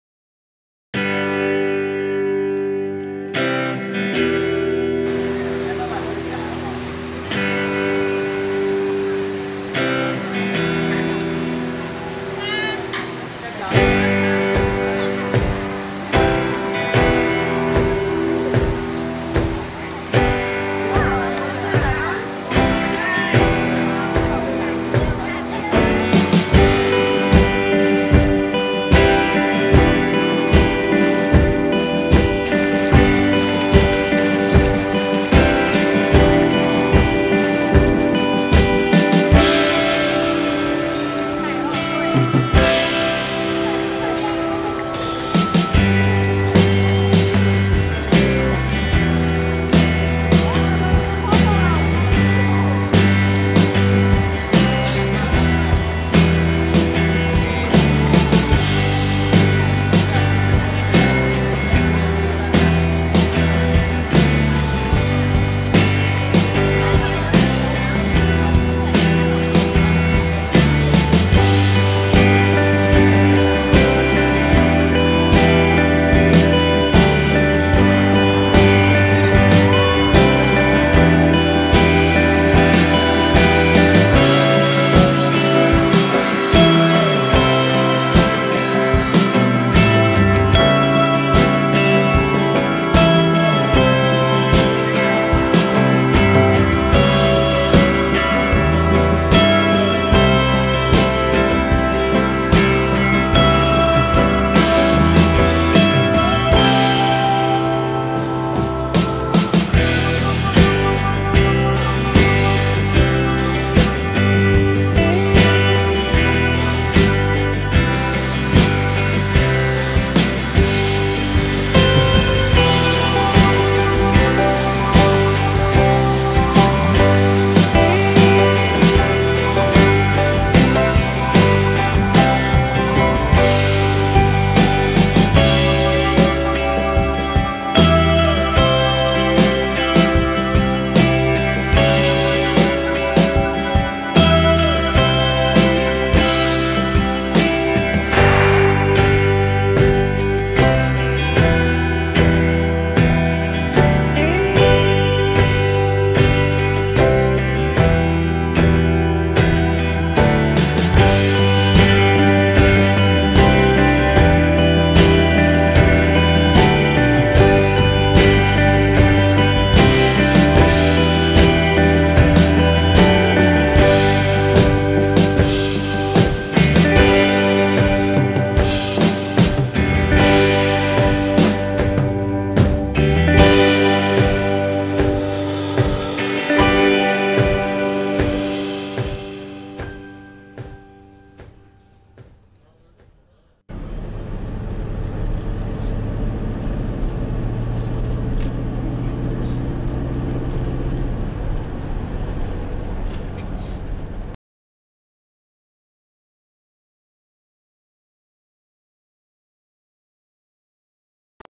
Walking around Singapore
This short video made from our holiday at Singapore.  We just been around and capture some interesting views. Enjoy the video.Thanks.